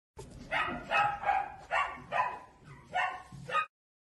Jack Russell
Jack Russell fiyatları yavruları
Jack Russell Terrier, sağlam yapılı ve atletik köpek Tilki Terrieri ırkının daha az rafine haline benzemektedir. En az % 51 i beyaz olmak kaydı ile özellikle baş ve kuyrukta kızılımsı siyah, ten rengi ve kahverengi lekeler görülür. Kafatası kulak arasında geniştir ve buruna doğru sivrilir. Alın çıkıntısı belirgin ancak çok keskin değildir. Burun siyah ve gözler koyu renkli ve badem şeklindedir. V şeklindeki kulaklar öne doğru kıvrıktır.